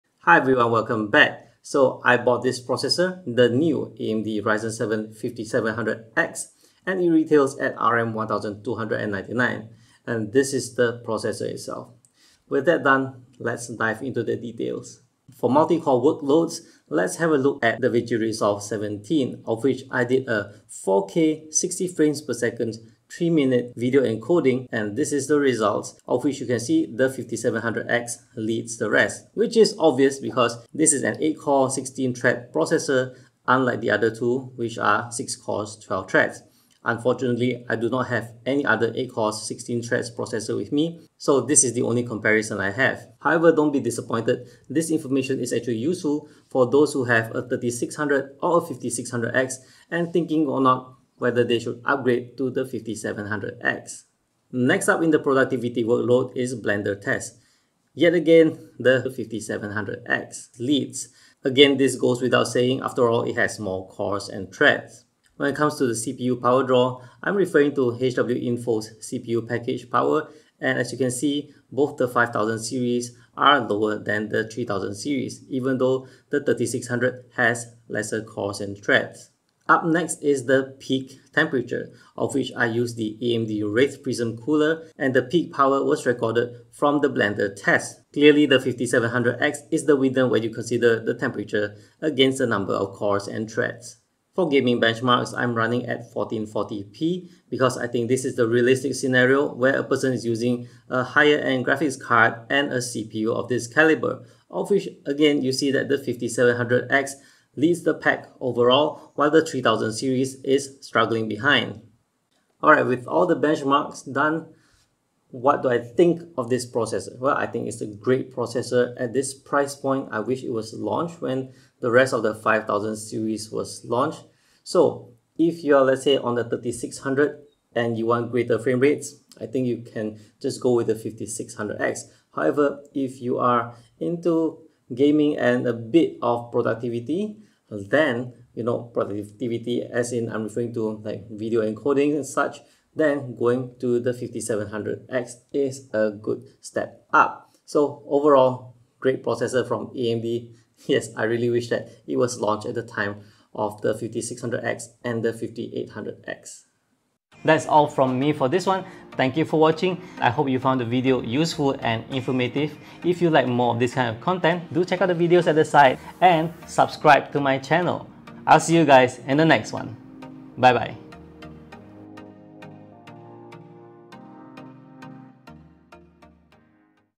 AMD Ryzen 7 5700X Review - I wish it was launched earlier
The AMD Ryzen 7 5700X just arrived and I got mine from retail and I am totally pleased with it.

Back when I first reviewed the Ryzen 5 5600X and Ryzen 7 5800X I wish there was a Ryzen 7 5700X, something at the same core count of the 5800X but in between the price of both of them and at lower power draw.

Right now after about 1.5 years later it's finally here, it's good but it's just too bad it wasn't launched earlier. Even so it's a really great processor and I'm very sure it makes the Ryzen 7 5800X less appealing.

0:00 I bought it, the Ryzen 7 5700X
0:15 Multi-core workloads - Davinci Resolve and Blender
1:03 CPU power draw
1:19 Temperature
1:34 Pushing framerates at 1440P, selected titles that show CPU variances easily
1:52 Summary